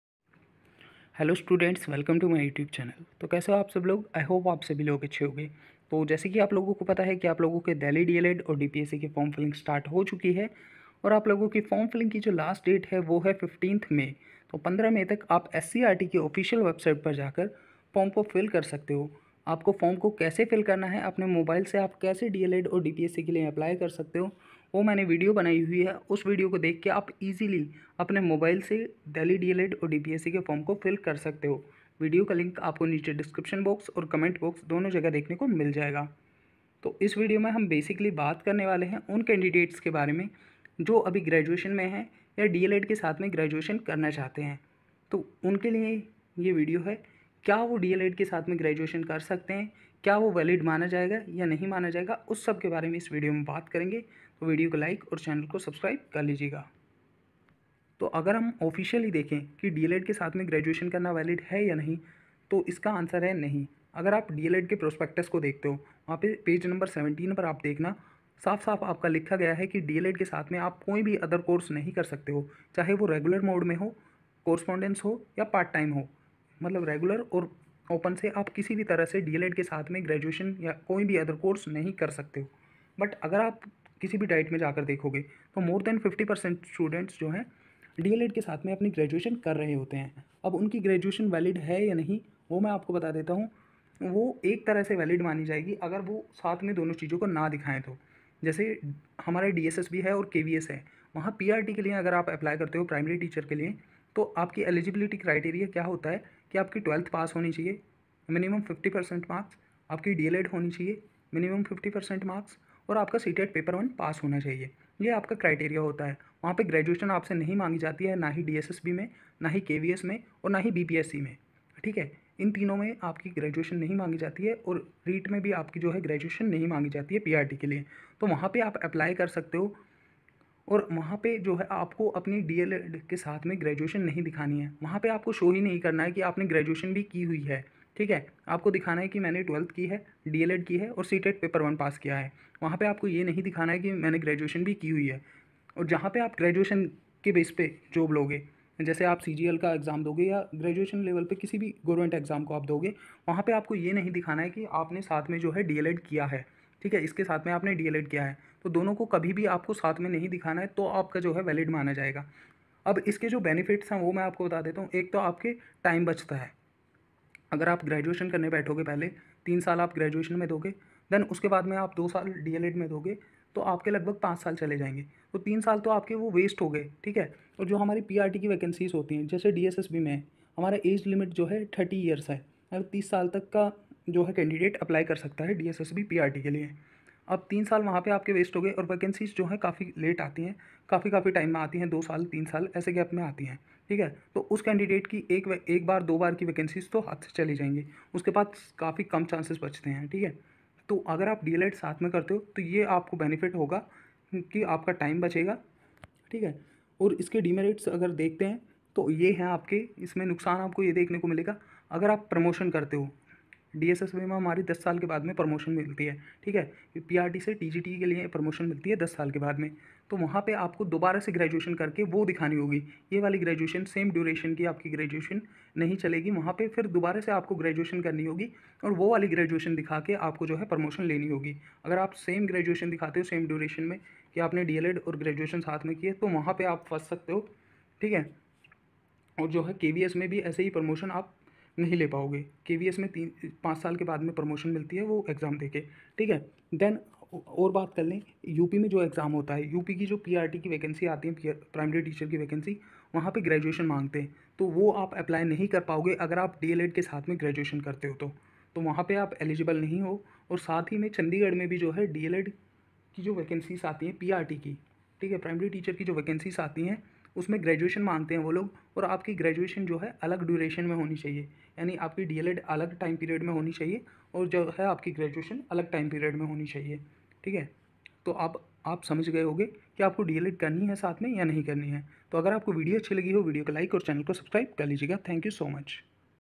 क्या GRADUATION के साथ D.EL.ED कर सकते हैं ? || D.El.Ed or B.A दोनों साथ में करे तो क्या होगा
🎯 D.El.Ed ka Form Filling कैसे करे  video link 🔗🔗🔗👇🏻👇🏻👇🏻